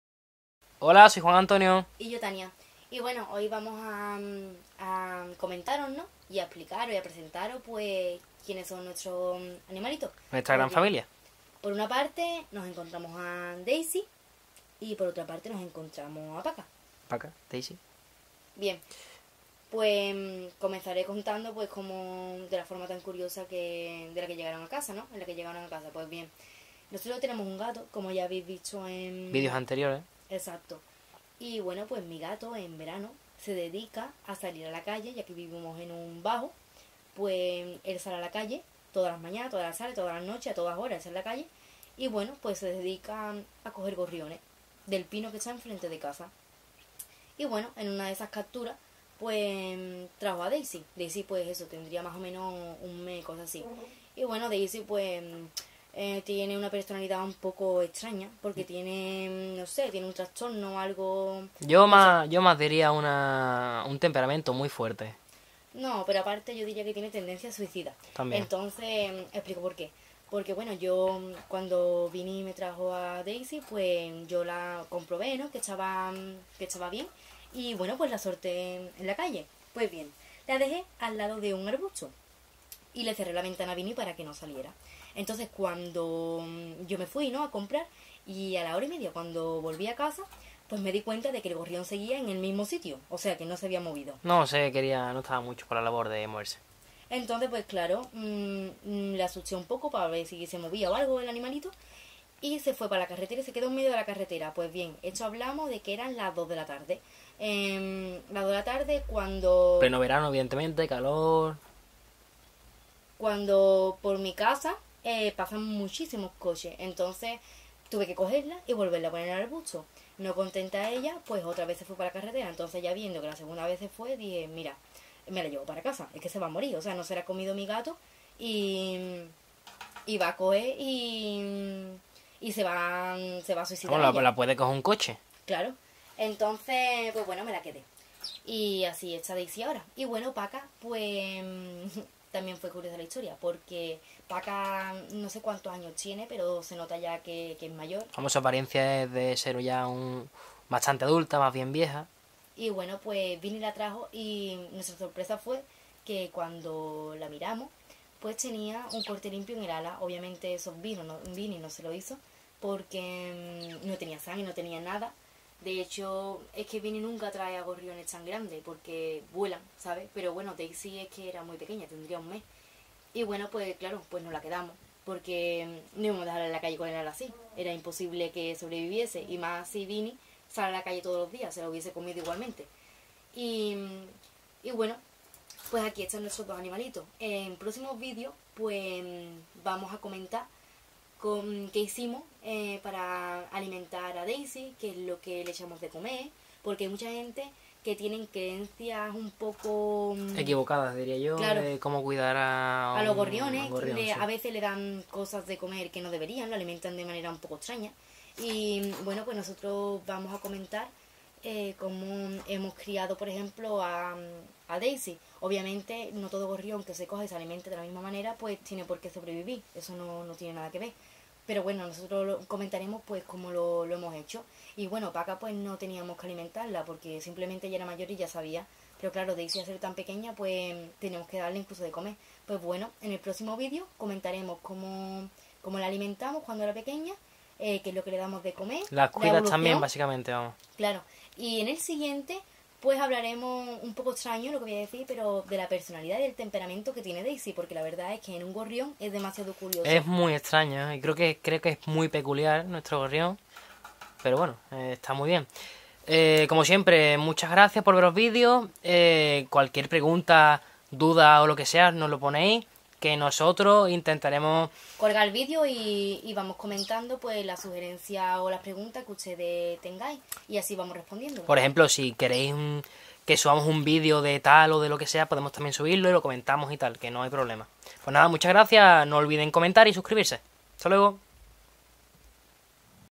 Gorriones como mascotas
En este nuevo vídeo presentamos a nuestros gorriones, Daisy y Paca. Comentaremos de la forma tan curiosa que llegaron a casa. Gracias.